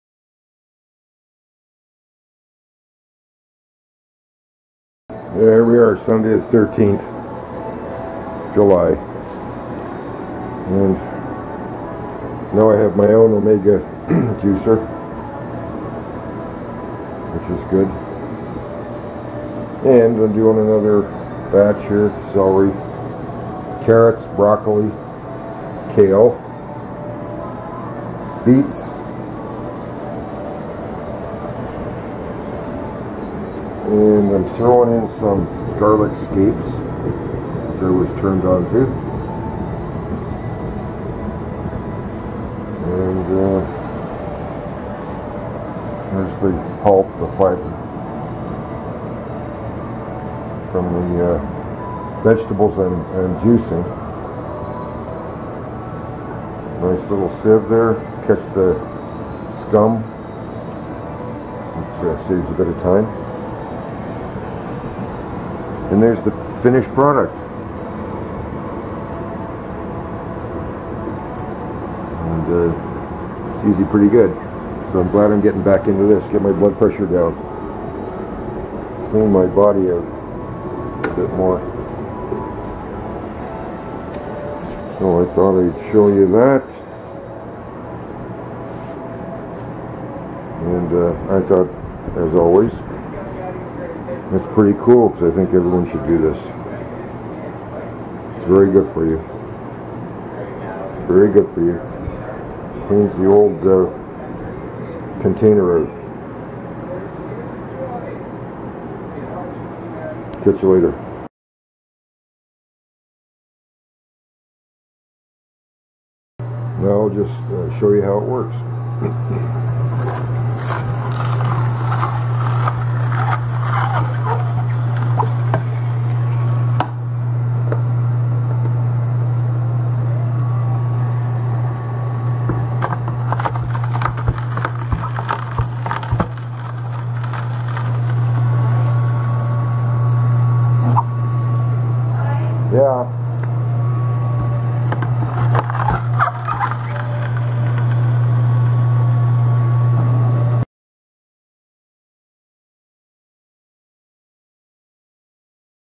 Juicing with the Omega
Just showing you a juicing session I did with my own Omega juicer. I did beets, carrots, broccoli, celery, kale and also added some garlic scapes.Time to clean out my system with the fresh squeezings of Mother Nature's bounty! I did up three mason jars of juice and used quite a few scapes in the last batch. And that last batch was GARLICKY! (If that's even a word!!). The garlic scapes are the immature flower of the garlic plant which are removed to allow all the nutrients to be used by the bulb in the ground. M.